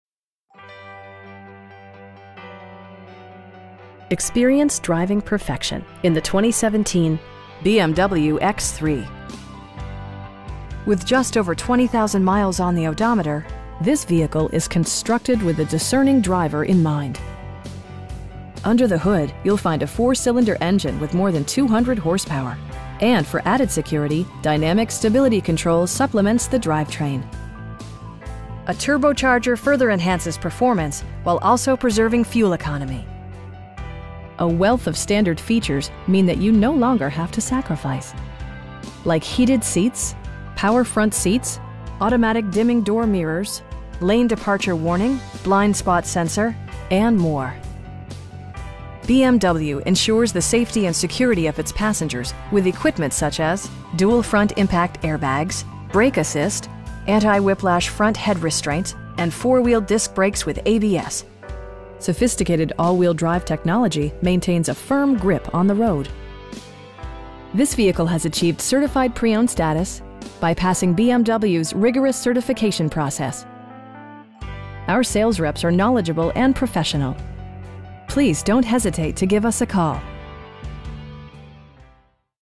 2017 BMW X3 xDrive28i Sports Activity Vehicle
Experience driving perfection in the 2017 BMW X3. With just over 20,000 miles on the odometer, this vehicle is constructed with the discerning driver in mind. Under the hood you'll find a 4 cylinder engine with more than 200 horsepower, and for added security, dynamic Stability Control supplements the drivetrain. A turbocharger further enhances performance, while also preserving fuel economy. A wealth of standard features means that you no longer have to sacrifice. Like heated seats, power front seats, an automatic dimming rear-view mirror, an outside temperature display, automatic dimming door mirrors, lane departure warning, blind spot sensor, and more. With high intensity discharge headlights illuminating your path, you'll always appreciate maximum visibility. For drivers who enjoy the natural environment, a power moon roof allows an infusion of fresh air. BMW ensures the safety and security of its passengers with equipment such as: dual front impact airbags, head curtain airbags, traction control, brake assist, anti-whiplash front head restraints, ignition disabling, an emergency communication system, and 4 wheel disc brakes with ABS. Sophisticated all wheel drive technology maintains a firm grip on the road. This vehicle has achieved Certified Pre-Owned status, by passing BMW's rigorous certification process. Our sales reps are knowledgeable and professional. We'd be happy to answer any questions that you may have. Please don't hesitate to give us a call.